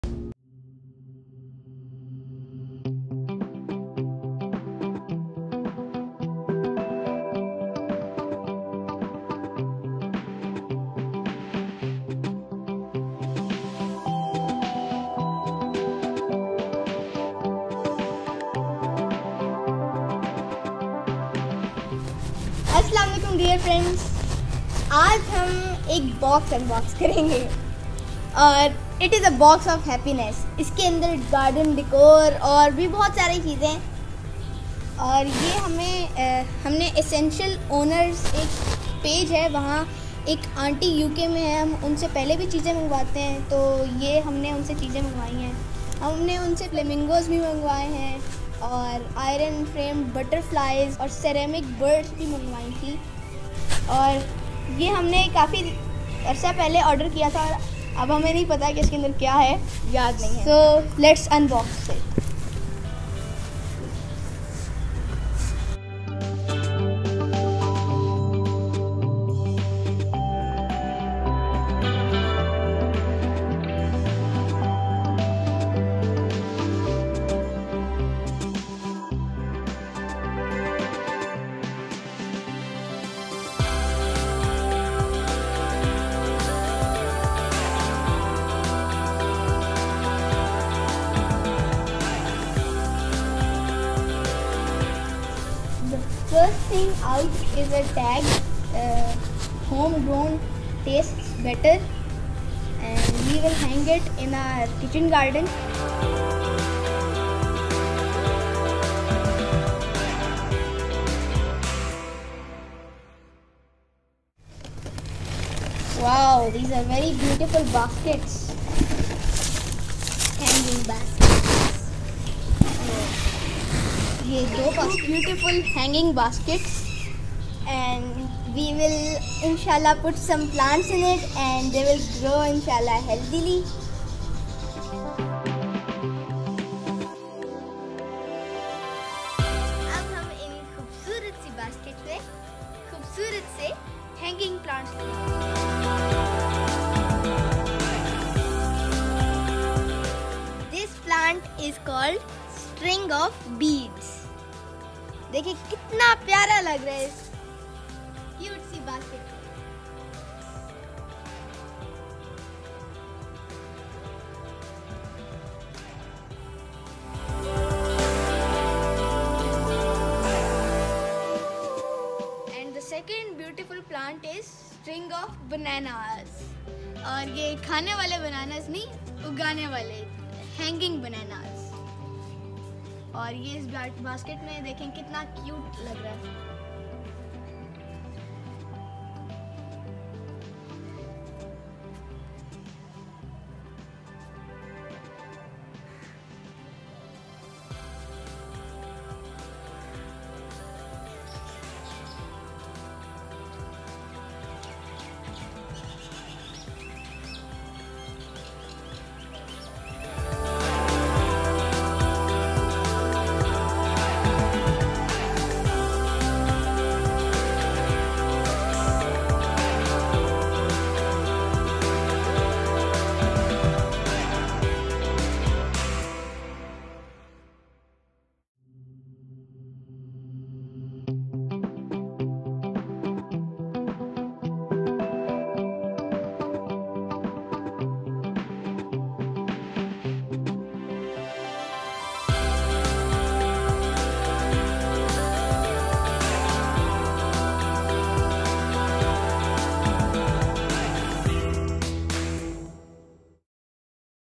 UNBOXING HAPPINESS, Beautiful Hanging Baskets
In this video, you will watch beautiful baskets and putting beautiful hanging plants in them , We Purchased from Essensuals Owner.